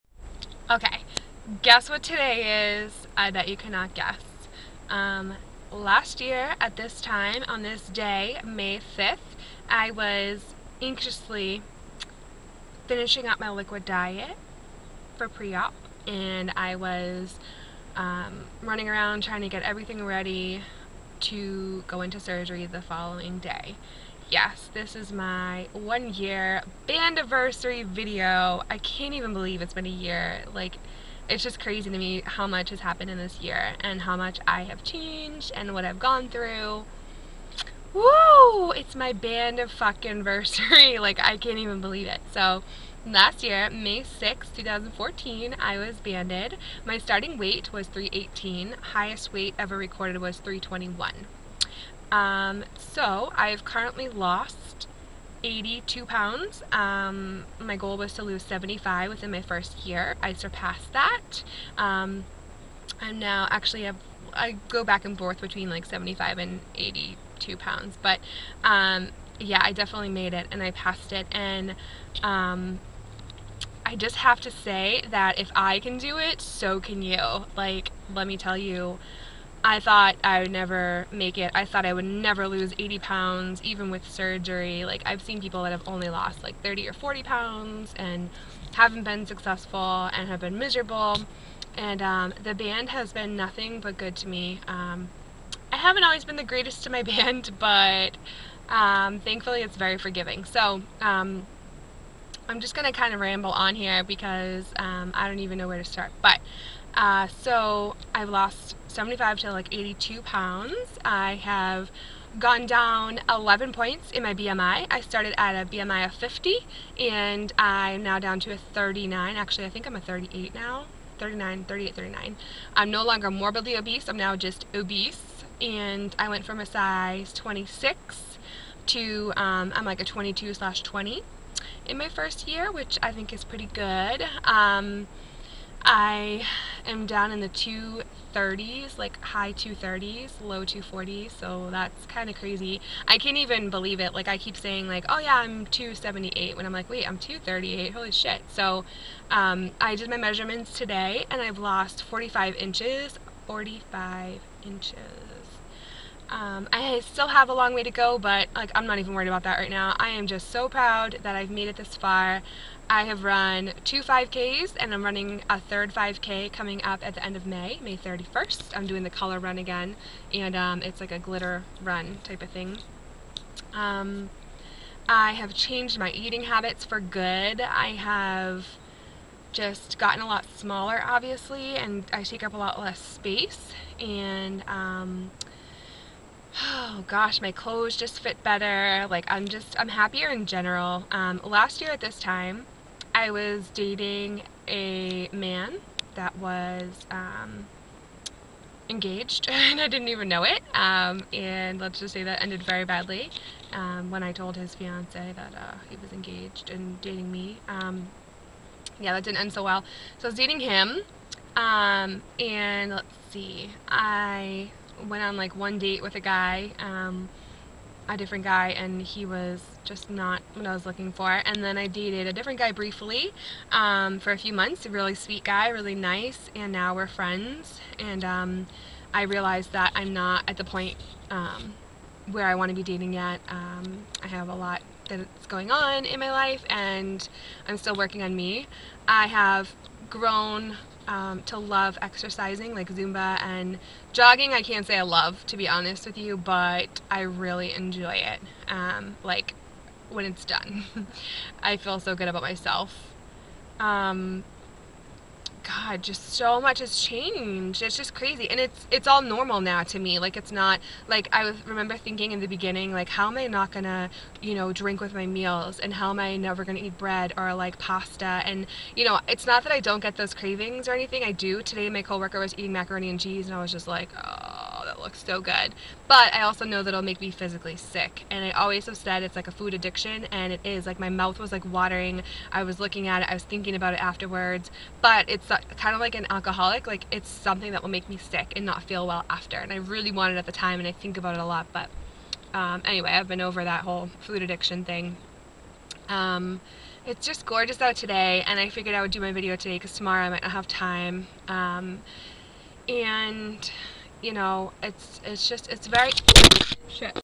Bandiversary!! One year post-op Lap Band!!!! (Part 1)
I cannot believe I made it to one-year post op!! Down 80 lbs, 45 inches, three pants sizes, two shirt sizes, and just a lot more fit!!